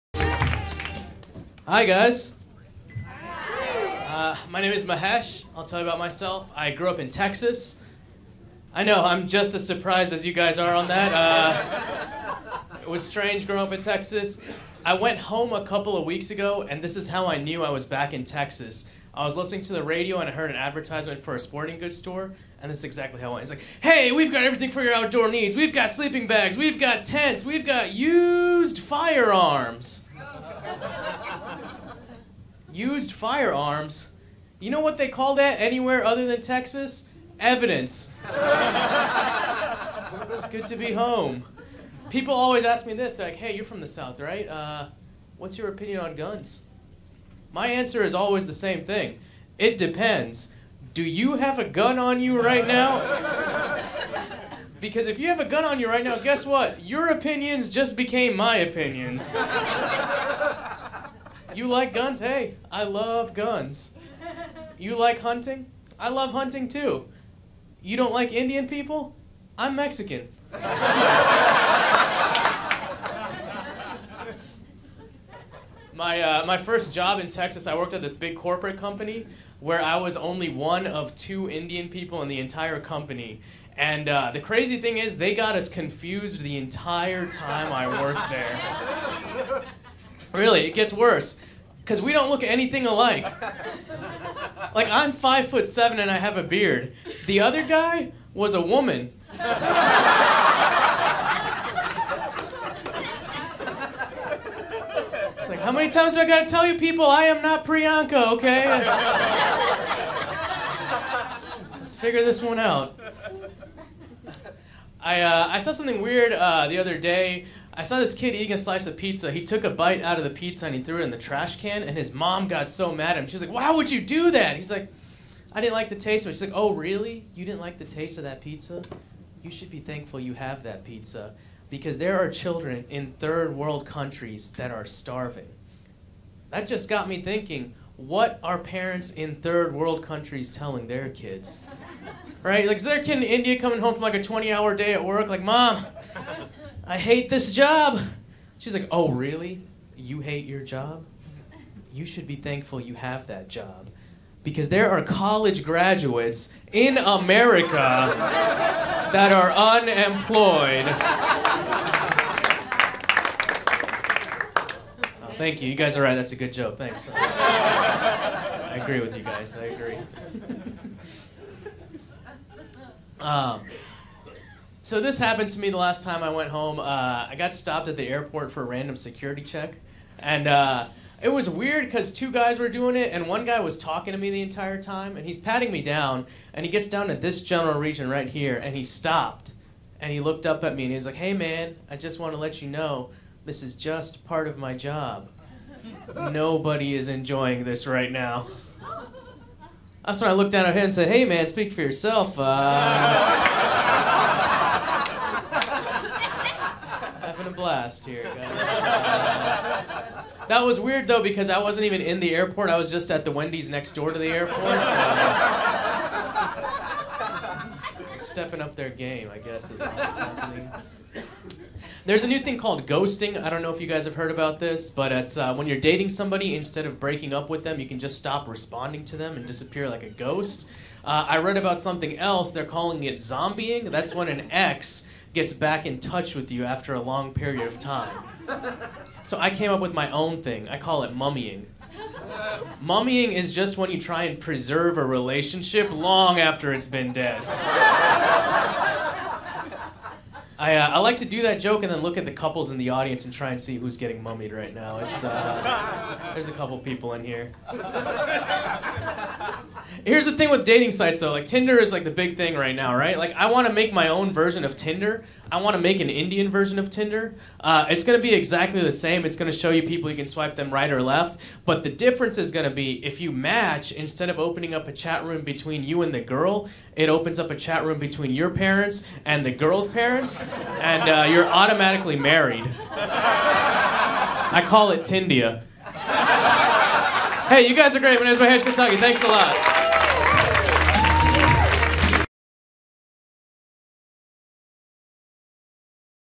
Mahesh Kotagi has trouble being an Indian from Texas
Mahesh Kotagi Live from Flappers Comedy Club & Restaurant located in lovely downtown Burbank: the entertainment Capital of the World.